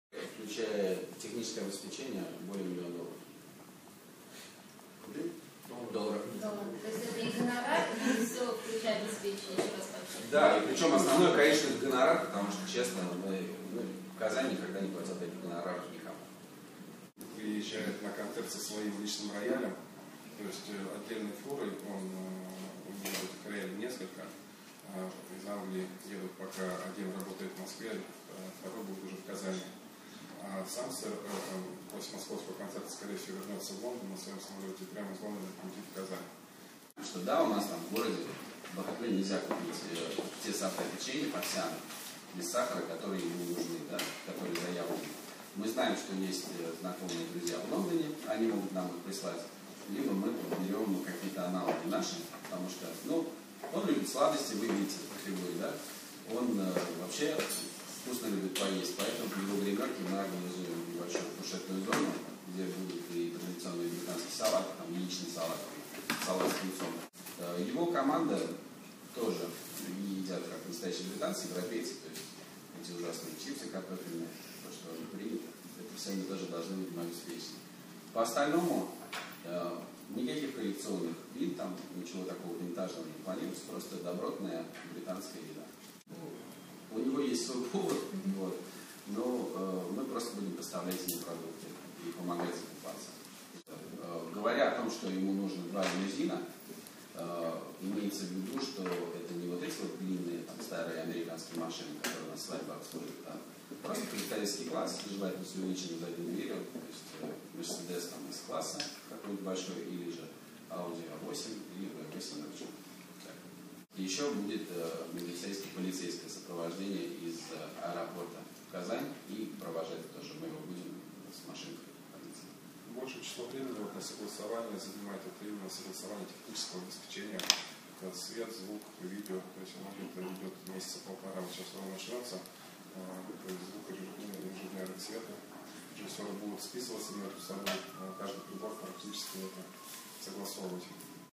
райдер Элтона Джона в Казани
Устроители концерта Элтона Джона в Казани  истратят на этот проект около $1 млн., львиная доля уйдет на выплату гонорара певцу.
Сэр Элтон Джон прибудет в Казань на личном самолете из Лондона в день концерта 7 декабря. Накануне он дает концерт в Москве, но ночевать в России не собирается, улетит в Лондон, откуда на другой день прибудет в Казань. За день до концерта в столицу Татарстана привезут рояль певца, он выступает только с личным инструментом. Таких роялей у него несколько, и они перемещаются между городами, в которых проходят гастроли.
Райдер певца скромен: здоровая британская еда и никаких коллекционных вин, вместе с певцом прибудет его повар. Основные проблемы -- это согласование технического оборудования, особенно звуковая аппаратура. Певец очень трепетно относится к звуковому ряду.